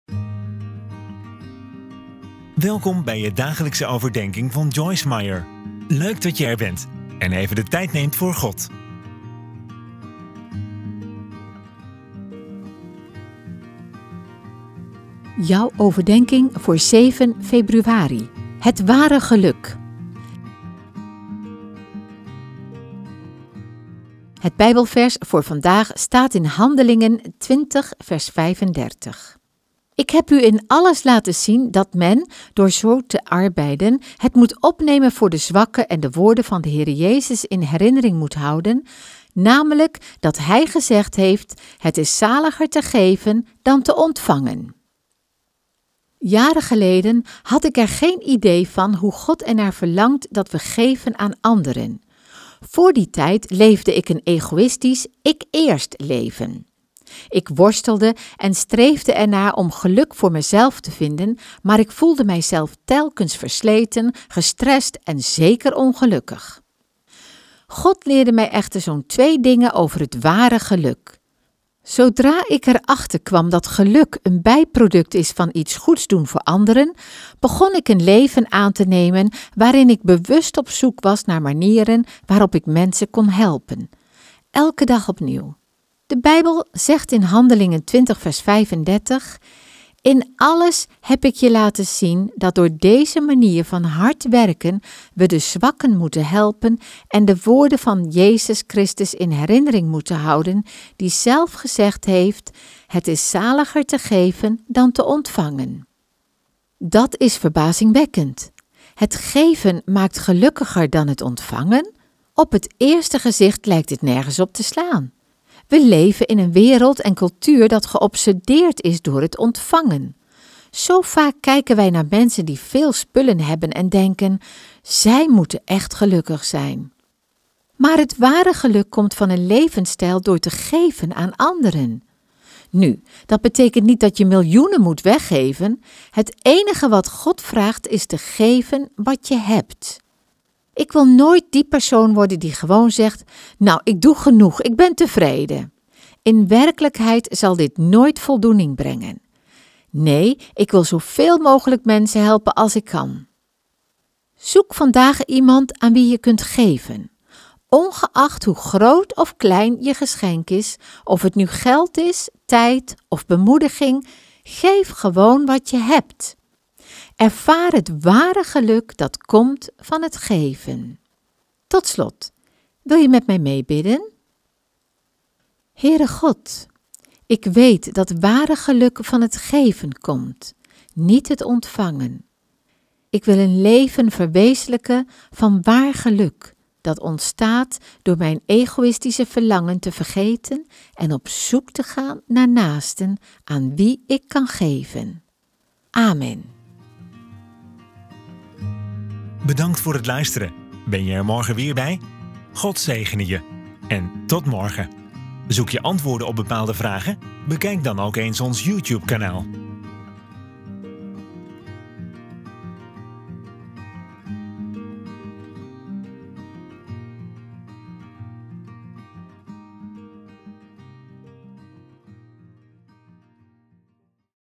Het ware geluk | Geven maakt blij | Overdenking | Joyce Meyer
Jaren geleden had ik er geen idee van hoe God ernaar verlangt dat we geven aan anderen. Voor die tijd leefde ik een egoïstisch, “ik eerst” leven. Ik worstelde en streefde ernaar om geluk voor mijzelf te vinden, maar ik voelde mijzelf telkens versleten, gestrest en zeker ongelukkig.

00:00 Dagelijkse overdenking van Joyce Meyer van 7 februari
00:15 Het ware geluk
00:25 Handelingen 20:35 (HSV)
Ik heb u in alles laten zien dat men, door zo te arbeiden, het moet opnemen voor de zwakken en de woorden van de Here Jezus in herinnering moet houden, namelijk dat Hij gezegd heeft: Het is zaliger te geven dan te ontvangen.
00:40 Overdenking

God leerde mij echter zo’n twee dingen over het ware geluk. Zodra ik er achter kwam dat geluk een bijproduct is van iets goeds doen voor anderen, begon ik een leven aan te nemen waarin ik bewust op zoek was naar manieren waarop ik mensen kon helpen - elke dag opnieuw.

De Bijbel zegt in Handelingen 20:35, In alles heb Ik je laten zien dat door deze manier van hard werken we de zwakken moeten helpen, en de woorden van de Jezus Christus in herinnering moeten houden, die zelf gezegd heeft: “Het is zaliger te geven, dan te ontvangen.”

Dat is verbazingwekkend. Het geven maakt gelukkiger dan het ontvangen? Op het eerste gezicht lijkt dit nergens op te slaan. We leven in een wereld en cultuur dat geobsedeerd is door het ontvangen. Zo vaak kijken we naar mensen die veel spullen hebben en denken: “Zij moeten echt gelukkig zijn.”

Maar het ware geluk komt van een leefstijl door te geven aan anderen. Nu, dat betekent niet dat je miljoenen moet weggeven. Het enige wat God vraagt is te geven wat je hebt.

Ik wil nooit die persoon worden die gewoon zegt: “Nou ik doe genoeg, ik ben tevreden.” In werkelijkheid zal dit nooit voldoening brengen. Nee, ik wil zo veel mogelijk mensen helpen als ik kan.

Zoek vandaag iemand aan wie je kunt geven. Ongeacht hoe groot of klein je geschenk is, of het nu geld is, tijd, of bemoediging, geef gewoon wat je hebt. Ervaar het ware geluk dat komt van het geven.

02:54 Kort gebed
God, ik weet dat ware geluk van het geven komt, niet het ontvangen. Ik wil een leven verwezenlijken van ware geluk, dat ontstaat door mijn egoïstische verlangen te vergeten en op zoek te gaan naar naasten aan wie ik kan geven.